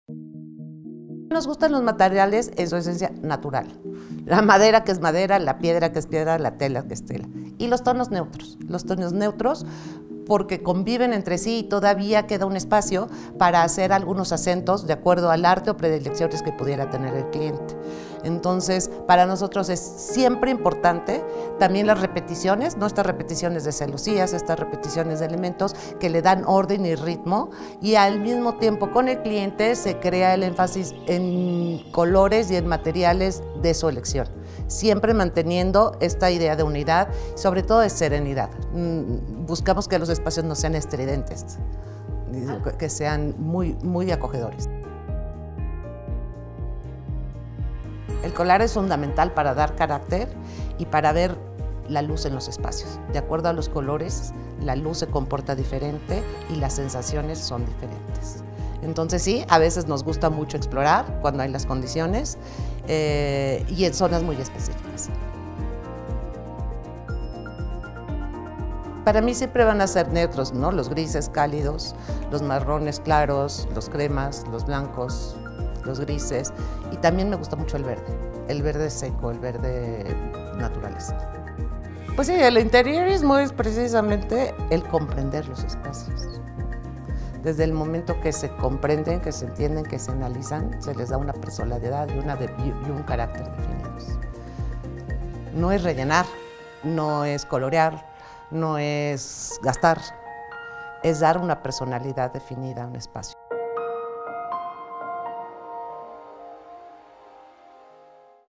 El interior que nos construye: Elsa Ojeda
La arquitecta Elsa Ojeda ostenta una amplia trayectoria en la arquitectura e interiorismo, por ello es que se declara como ‘una amante del diseño’.